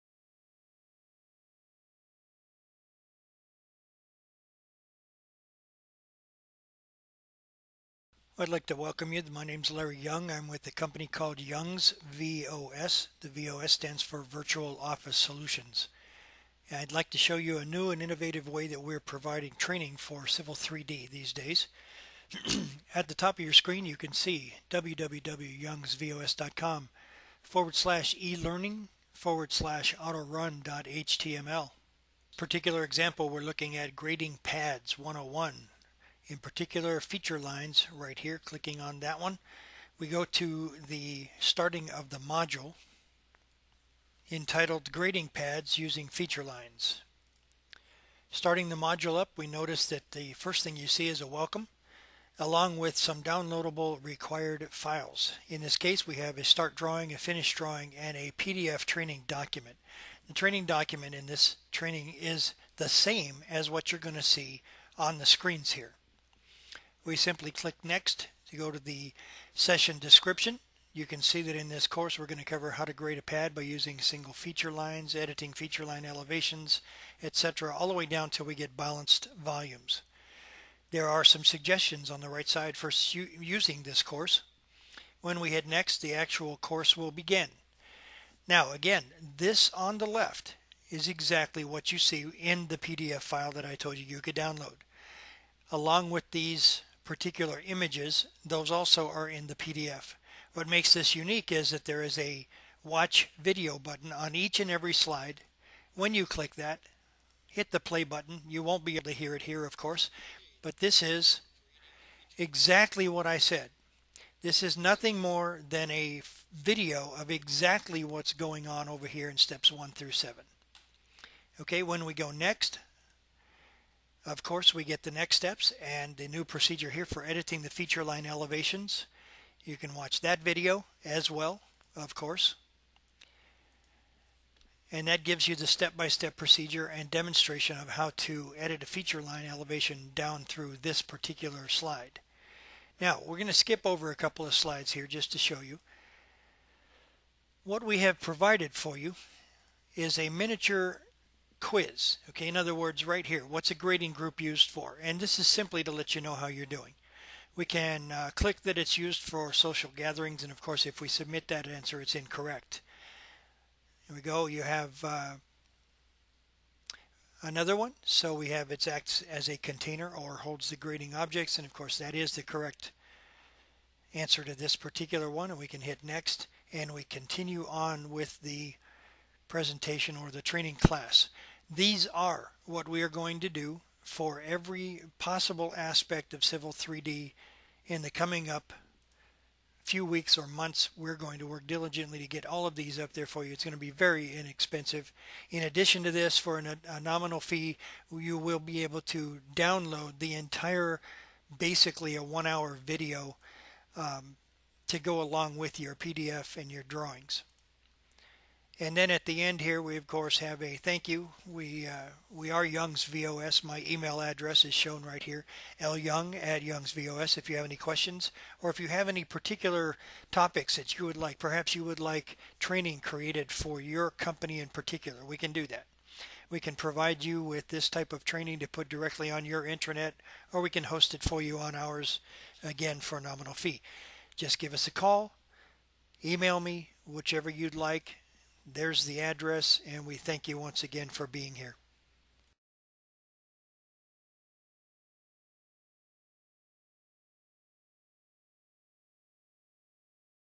U-Tube V-Lessons Promo
Young's VOS presents our newest method of Civil 3D Instruction.  V-Lessons are on-line, always available, and a much less costly training investment.  Each class is available with drawings and training manuals to download, and each page has a video provided to show you exactly how to accomplish the task at hand just as if you were in an instructor led live training class.